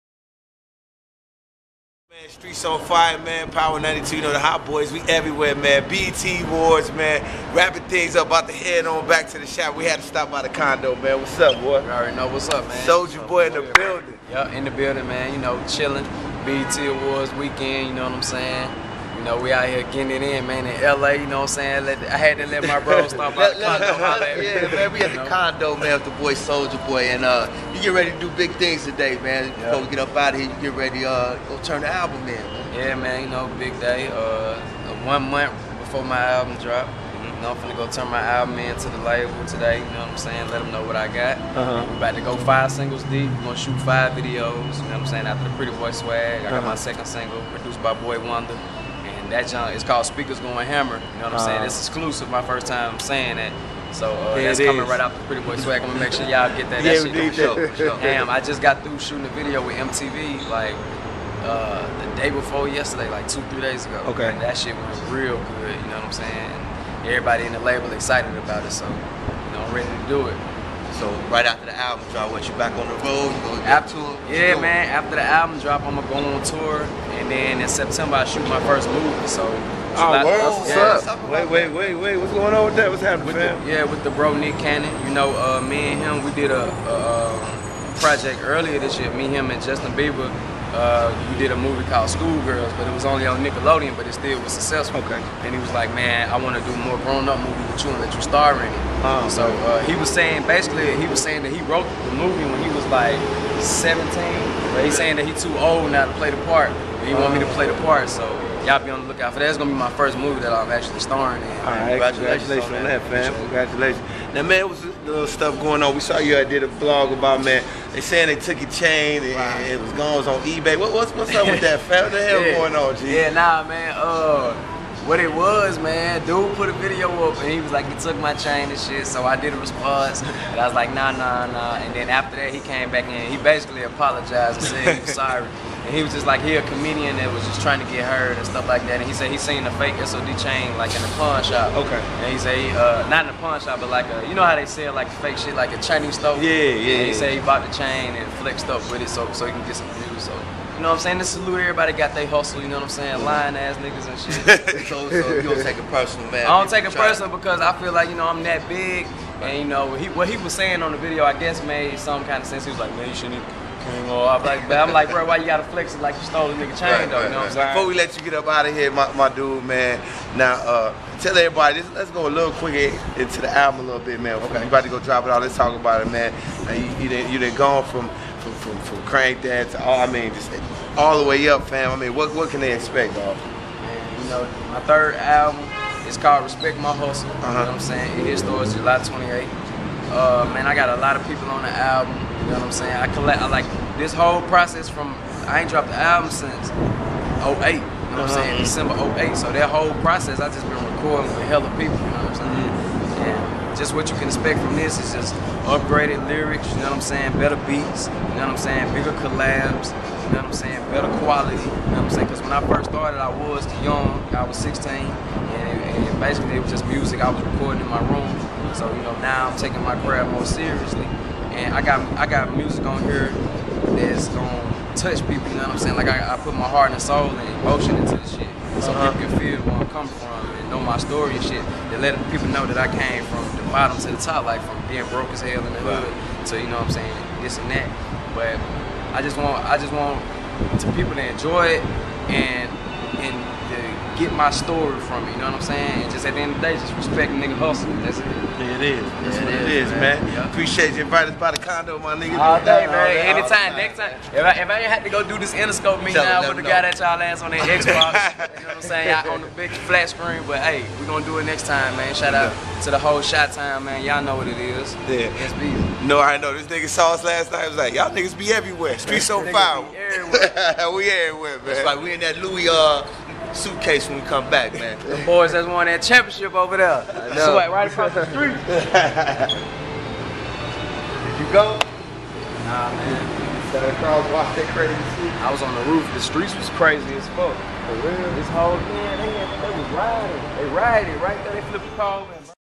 SOULJA BOY TALKS NEW ALBUM CHAIN STEALING WITH TONE AND SHAG
CHECK THE SOULA BOY INTERVIEW FROM THE CONDO ..BEFORE HE TURNS IN HIS NEW ALBUM !